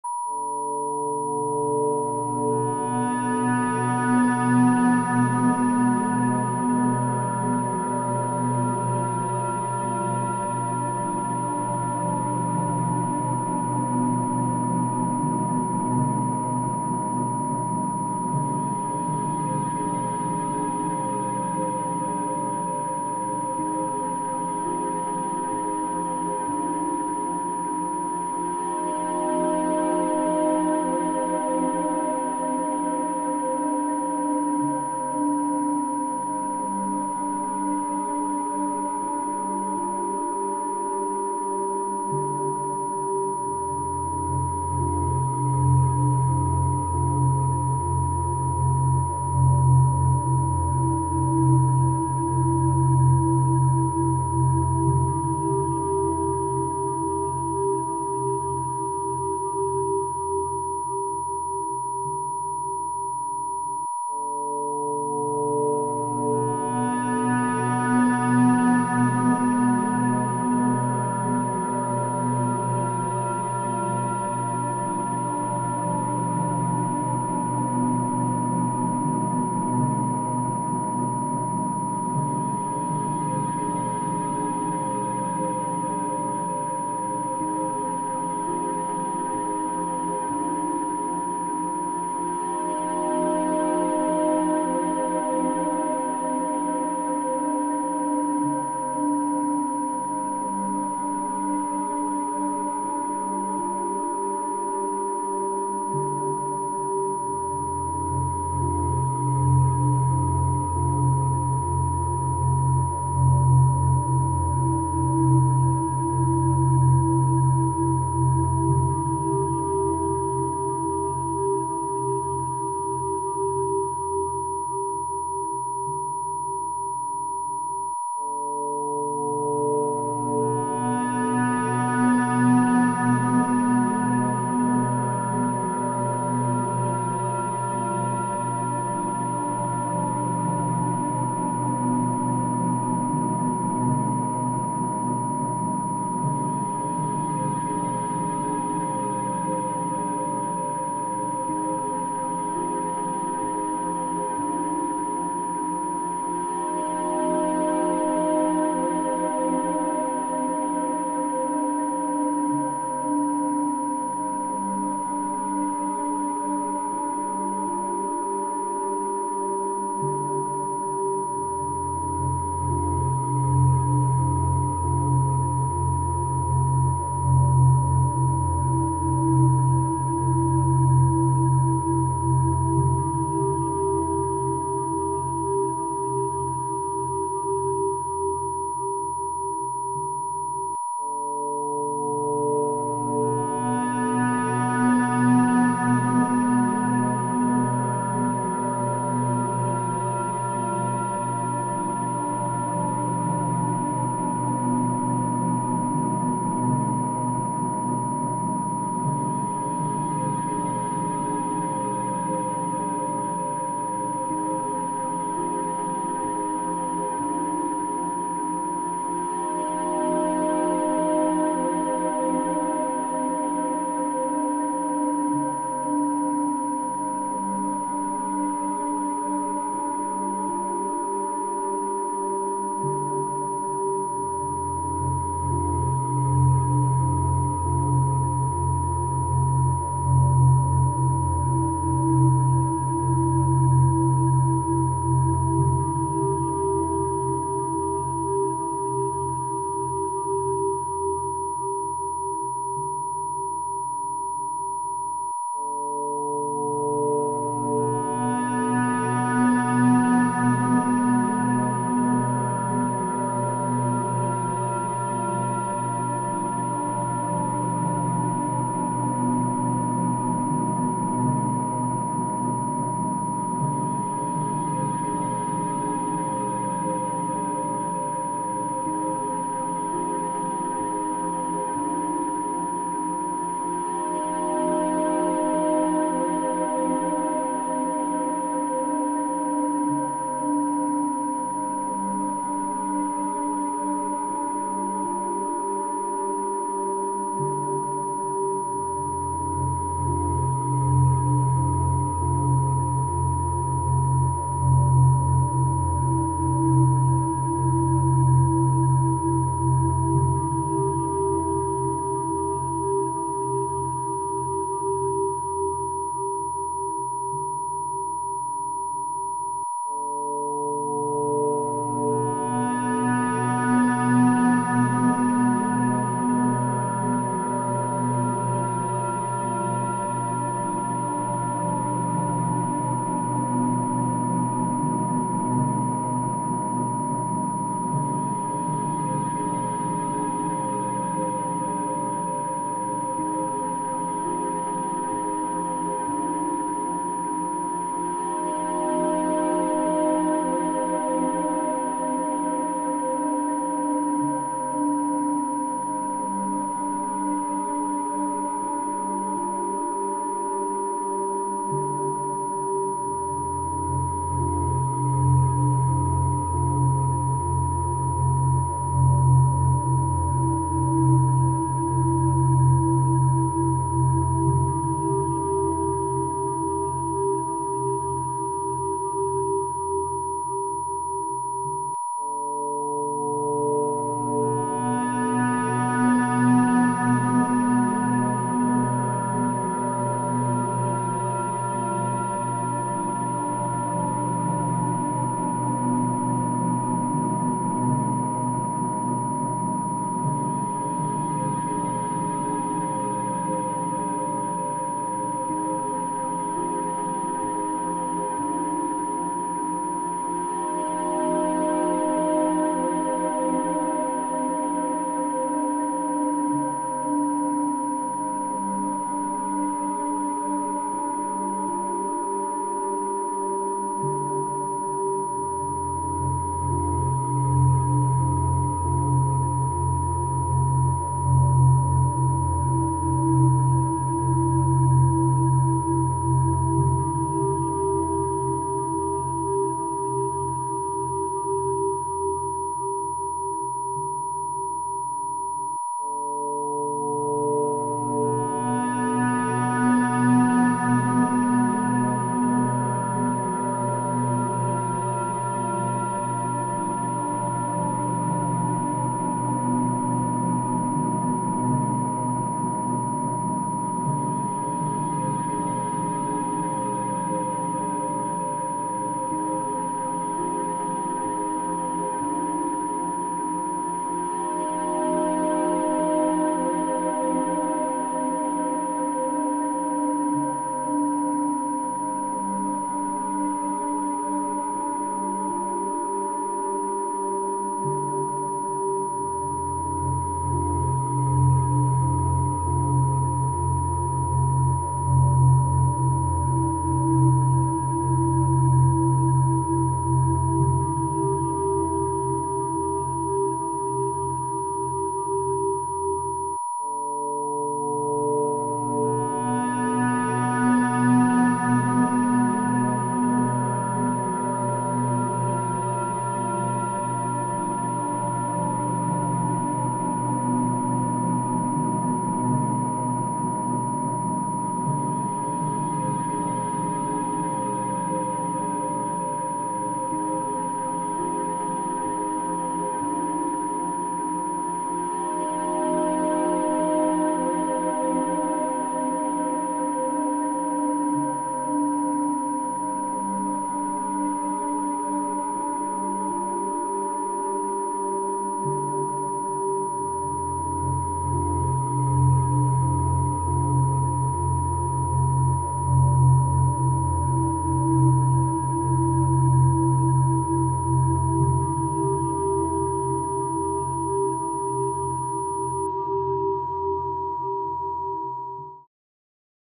Ancient Egypt Music, Activate Third Eye, Eye of Horus (10000Hz + 963Hz)
I played around with some frequencies and created this third eye activator.

When listening to frequencies, you should always drink enough water! The frequencies can be listened with speakers as well as with earphones.